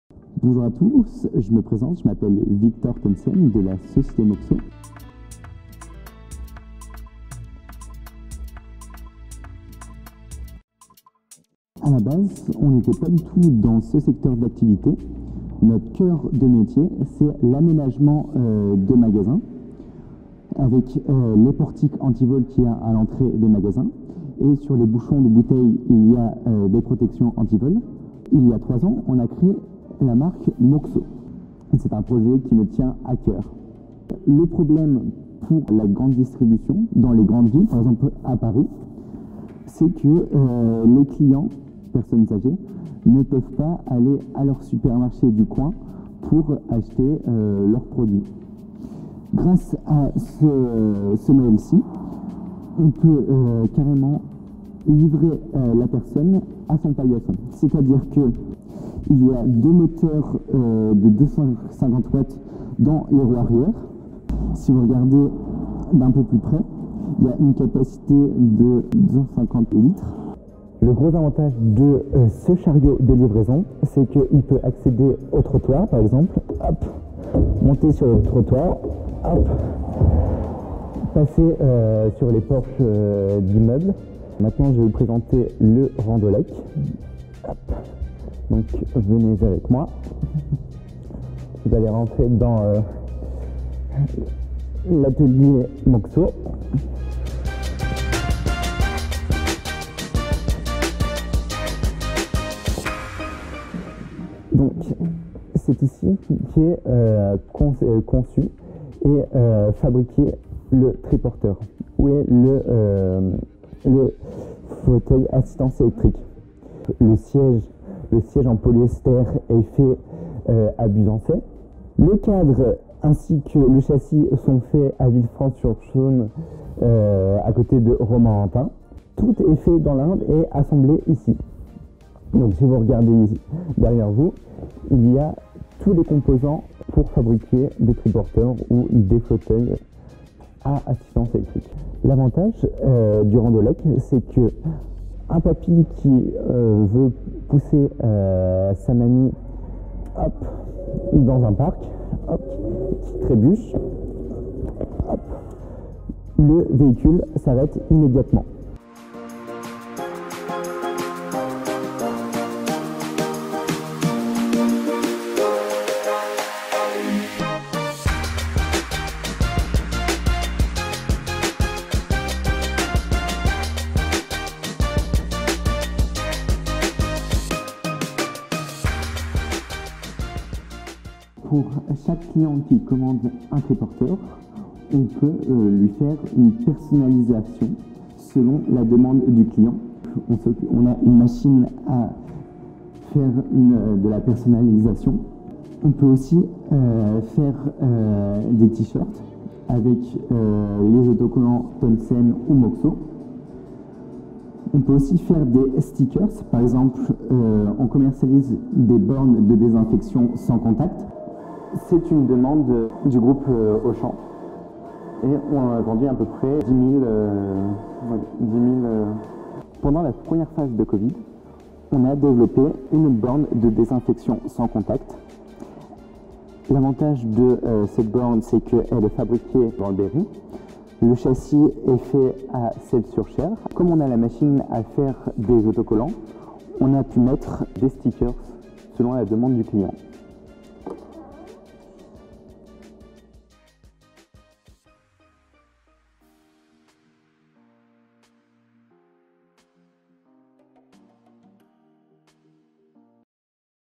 Présentation de Moxo par Victor Thoonsen
Cette semaine, on retrouve Victor Thoonsen qui nous présente le procédé de fabrication de ces produits adaptés pour toutes les personnes qui ont des difficultés à se déplacer. En prime, il nous présente des distributeurs de gel hydro alcoolique made in Indre ! L’occasion de rappeler l’importance des gestes barrières !

Prenez soin de vous et n'hésitez pas passer voir sa boutique et son atelier avenue Pierre de Coubertin !

Au Centre de la Question - une web-émission 100% Indre !

Préparation et montage : Lilou
Prise de vue : Thomas